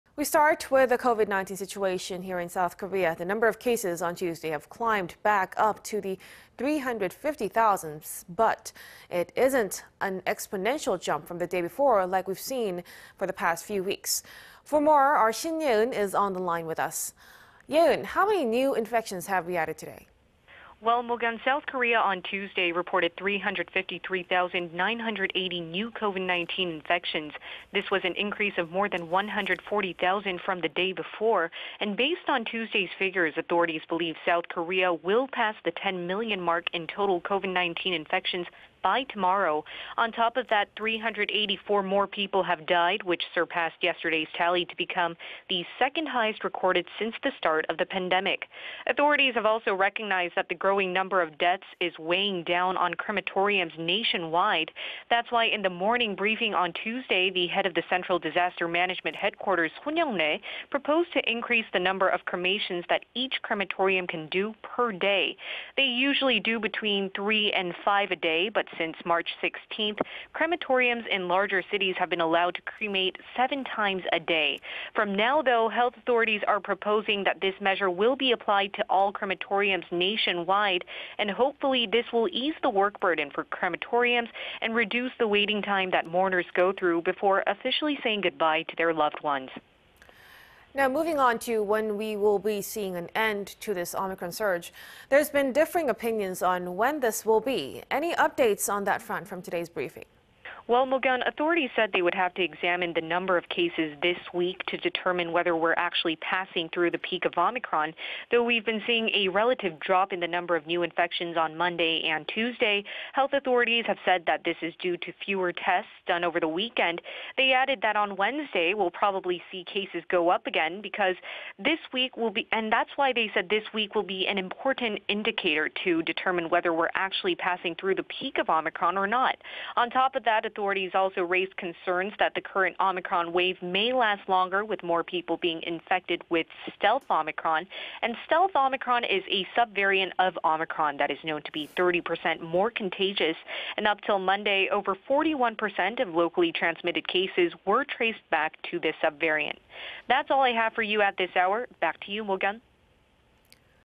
S. Korean authorities believe wave of Omicron may last longer due to spread of 'stealth Omicron'
확진 35만명대 급증..."유행 길어질 수도"

We start with the COVID-19 situation here in Korea.
The number of cases on Tuesday have climbed back up to the 350-thousands... but it isn't an exponential jump from the day before like we've been seeing for the past few weeks.
For more, Shin Ye-eun is on the line with us.
Ye-eun, how many new infections have we added today?

Mok-yeon... South Korea on Tuesday reported 353-thousand 9-hundred 80 new COVID-19 infections.
This was an increase of more than 140-thousand from the day before.
Based on Tuesday's figures... authorities believe South Korea will pass the 10-million mark in total COVID-19 infections by tomorrow.
On top of that, 3-hundred 84 more people died... which surpassed yesterday's tally to become the second-highest recorded since the start of the pandemic.
And authorities have recognized that the growing number of deaths is weighing down on crematoriums nationwide.
That's why in the morning briefing on Tuesday...the head of the Central Disaster Management Headquarters Son Young-rae proposed to increase the number of cremations that each crematorium can do per day.
They usually do between 3 and 5 cremations a day, but since March 16th, crematoriums in larger cities have been allowed to cremate 7 bodies a day.
From now though... health authorities are proposing that this measure will be applied to ALL crematoriums NATIONWIDE.
Hopefully... this will ease the work burden for crematoriums...and reduce the waiting time that mourners go through before officially saying goodbye to their loved ones.

Moving on to when we will be seeing an end to this Omicron surge. There's been differing opinions on when this will be. Any updates on that front from today's briefing.

Mokyeon...
Authorities said they would have to examine the number of cases THIS week...to determine whether we are ACTUALLY passing through the peak of Omicron.
Though we've been seeing a relative drop in the number of new infections on Monday and Tuesday... health authorities have said this is due to fewer tests done over the weekend.
They added... that on Wednesday we will probably see cases go up again.
That's why they said this week will be an important indicator to determine whether we're actually passing through the peak of Omicron or not.
On top of that... authorities also raised concerns that the current Omicron wave may last longer... with more people being infected with "stealth Omicron."
"Stealth Omicron" is a subvariant of Omicron that is known to be 30 percent more contagious.
Up till Monday, over 41 percent of locally transmitted cases were traced back to this subvariant.
That's all I have for you at this hour.
Back to you Mok-yeon.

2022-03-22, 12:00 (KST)